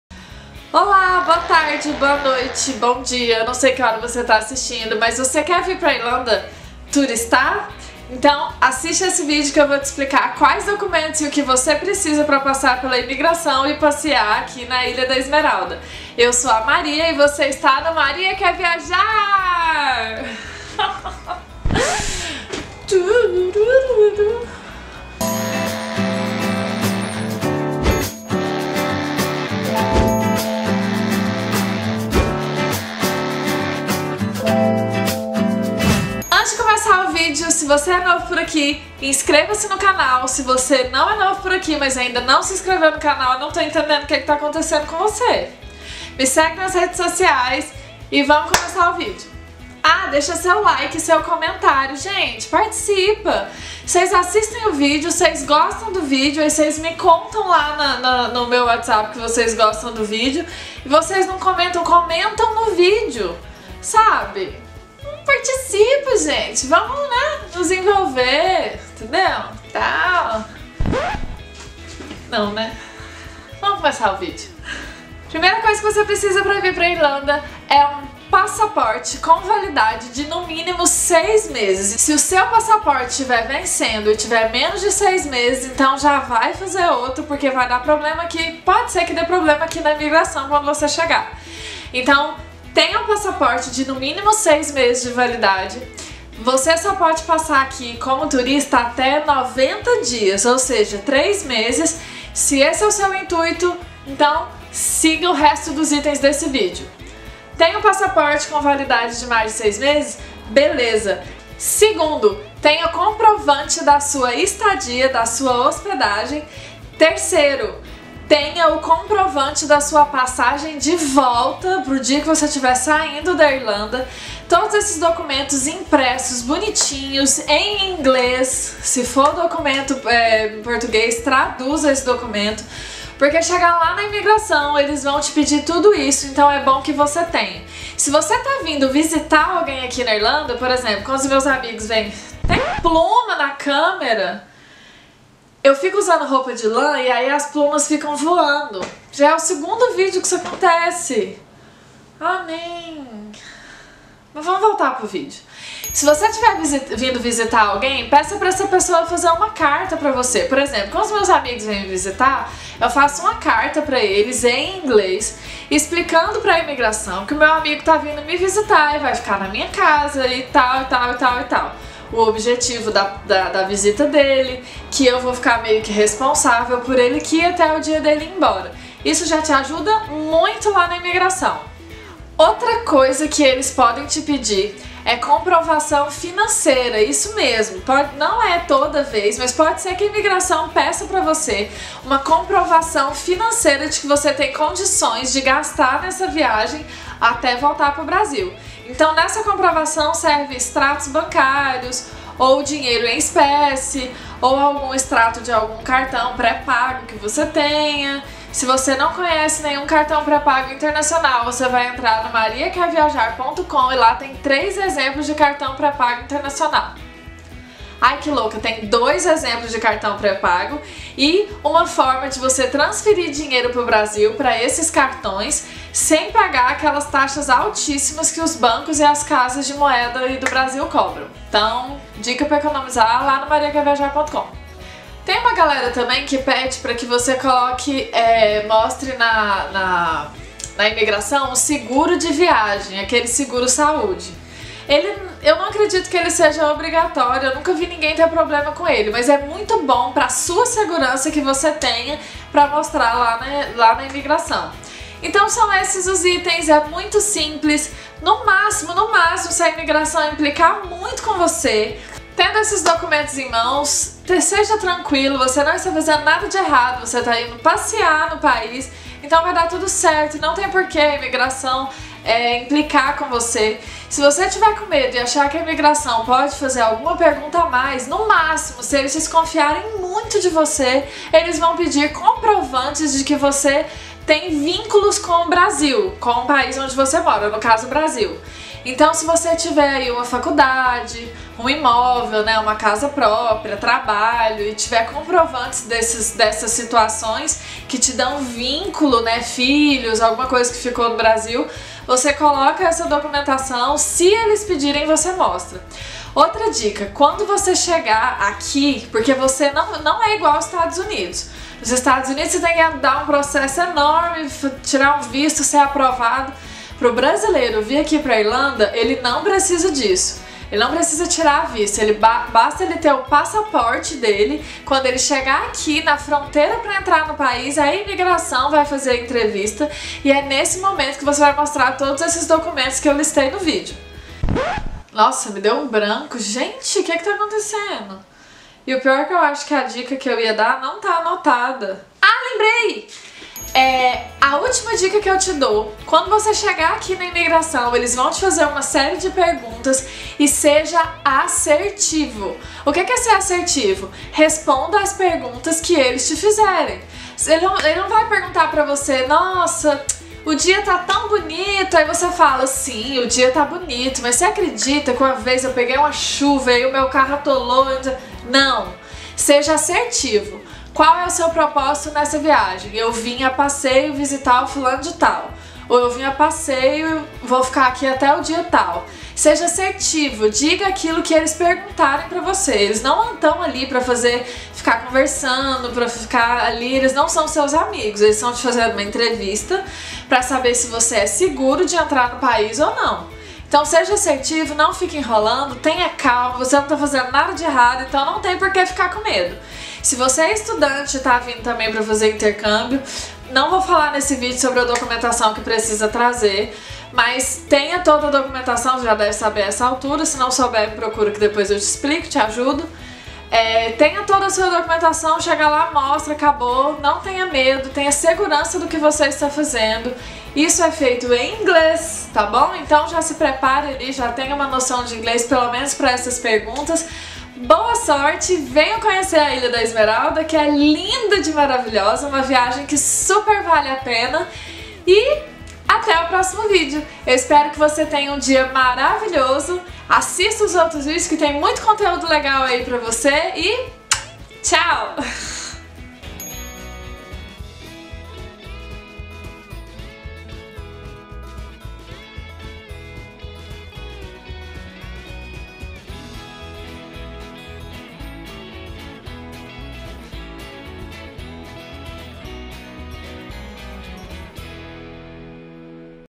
VOCÊ QUER VIR PRA IRLANDA TURISTAR!? - MQV #16
Espero que essas dicas ajude vocês que estão pensando em viajar  aqui para Irlanda!

Tendo os documentos certos e usando abordagem correta, não tem erro!

Se ajudou, comenta aí para que eu saiba!

Conheça mais das minhas divertidas histórias:

💡Produção e direção de arte: Jimmy Anthony (@jimmyanth / YouTube.com/jimmyanthony), diretor Impetus Produtora 🚀